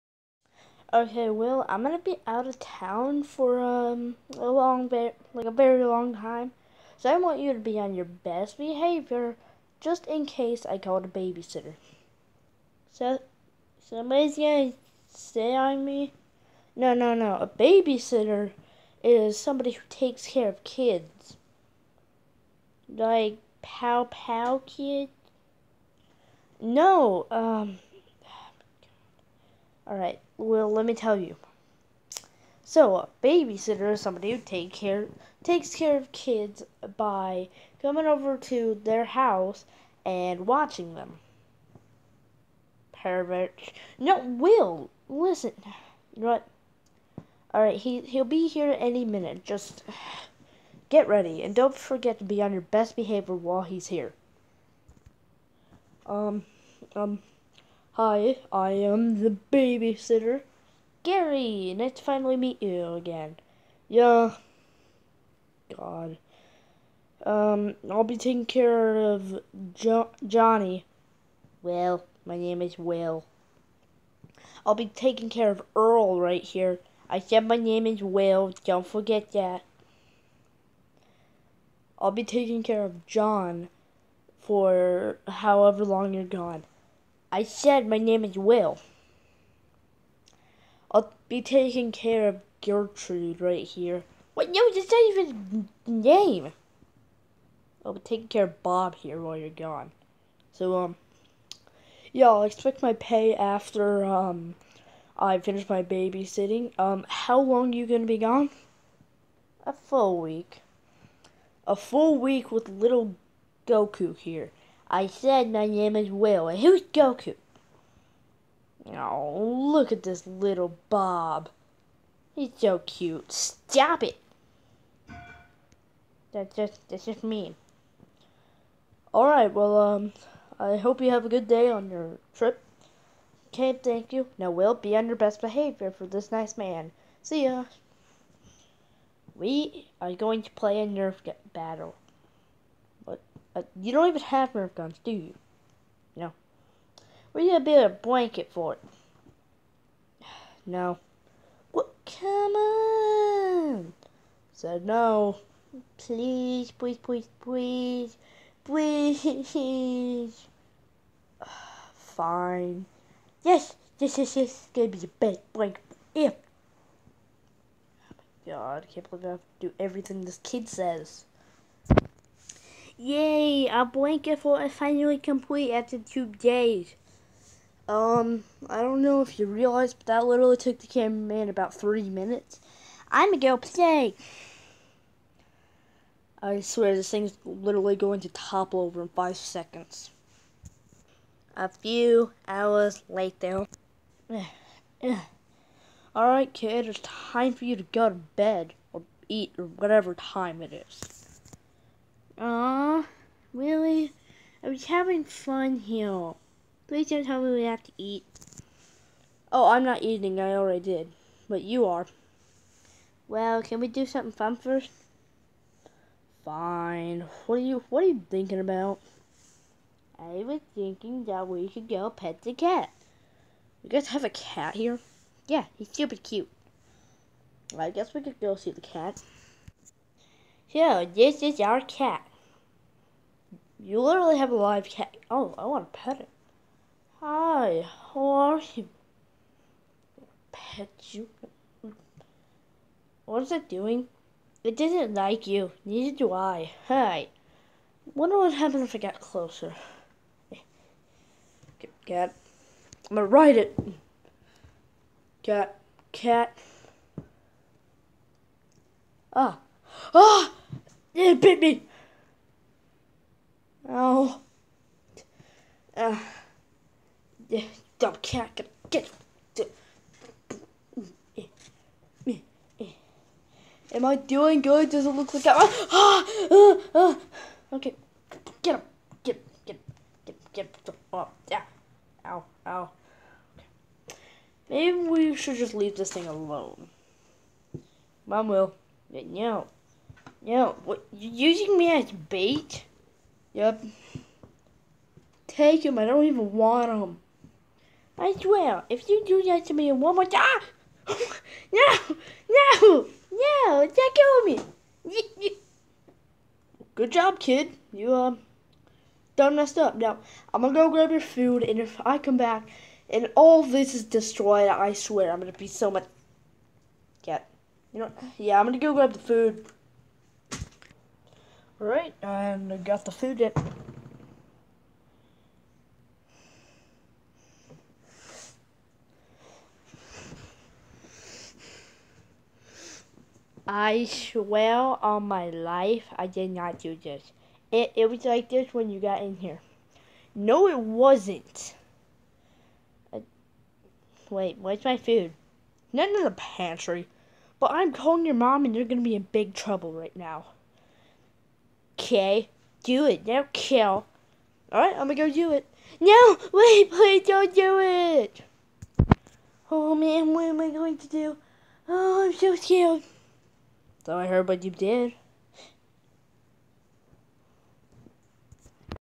WPA Short: Will's Babysitter!
Will gets babysat by none other than Gary!